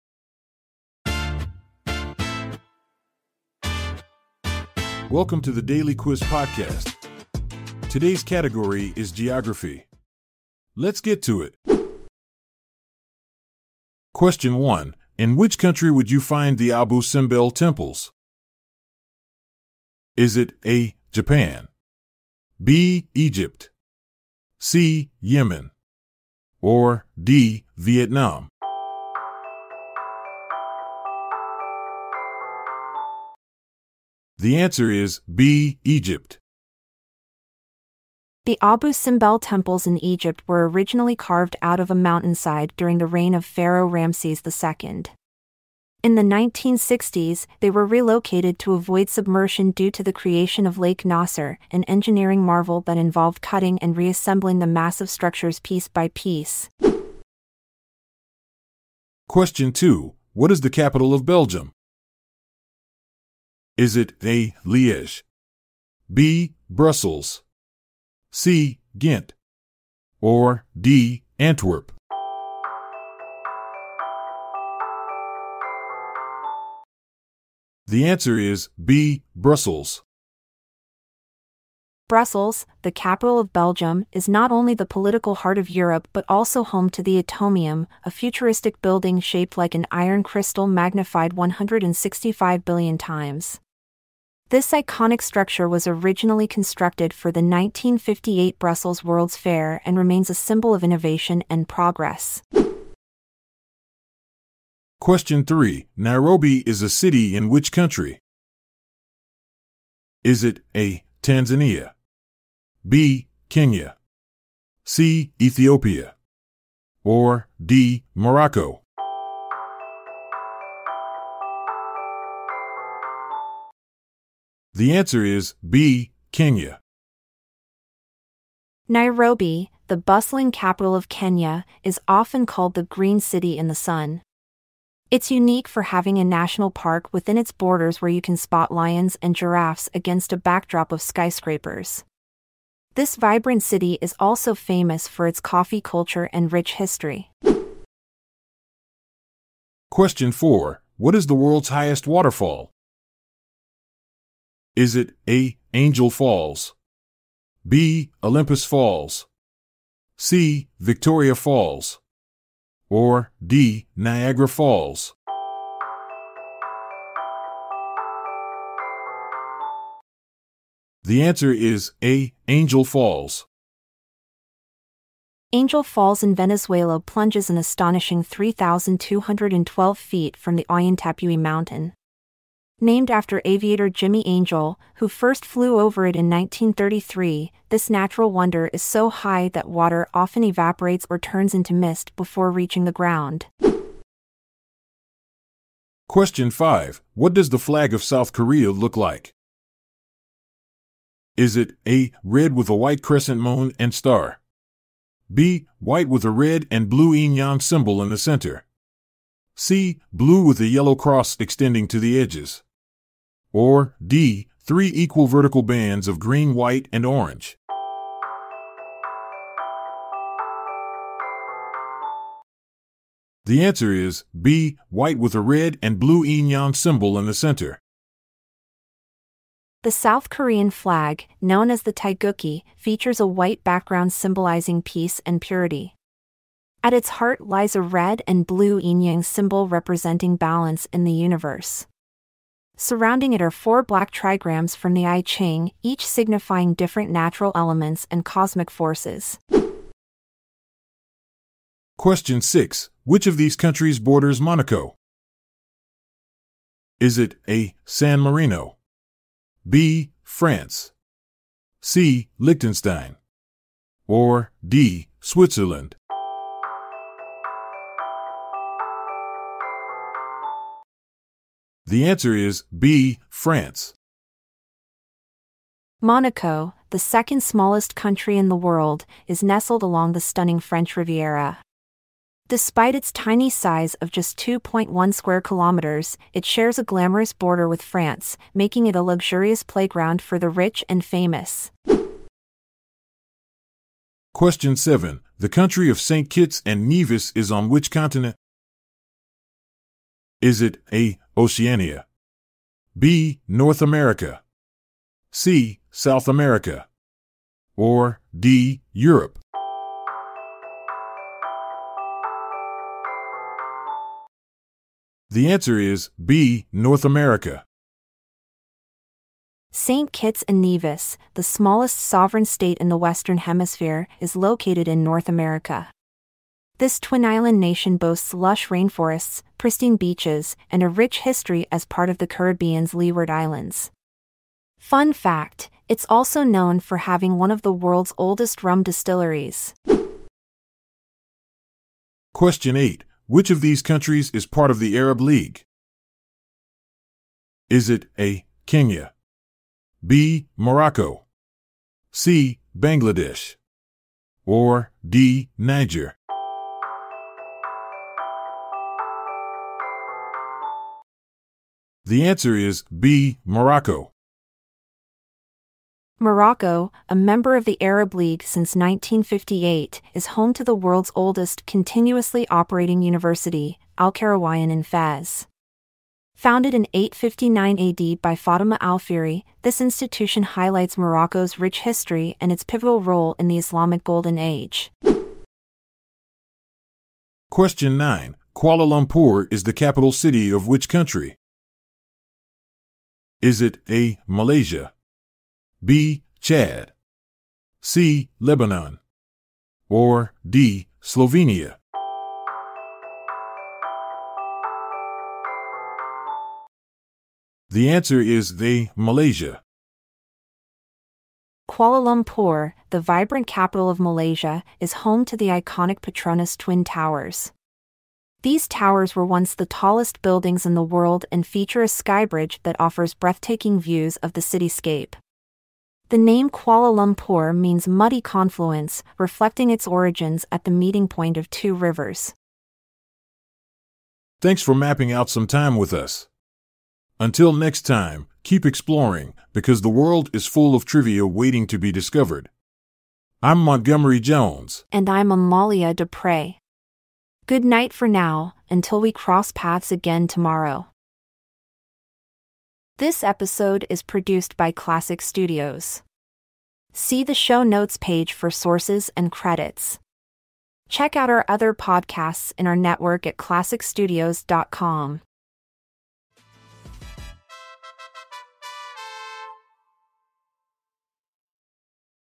Geography | In which country would you find the Abu Simbel temples? (+ 8 more...)
The Daily Quiz - Geography

 Today's Questions:

  •  Question 1: In which country would you find the Abu Simbel temples?

  •  Question 2: What is the capital of Belgium?

  •  Question 3: Nairobi is a city in which country?

  •  Question 4: What is the world's highest waterfall?

  •  Question 5: What does the flag of South Korea look like?

  •  Question 6: Which of these countries borders Monaco?

  •  Question 7: The country of Saint Kitts and Nevis is on which continent?

  •  Question 8: Which of these countries is part of the Arab League?

  •  Question 9: Kuala Lumpur is the capital city of which country?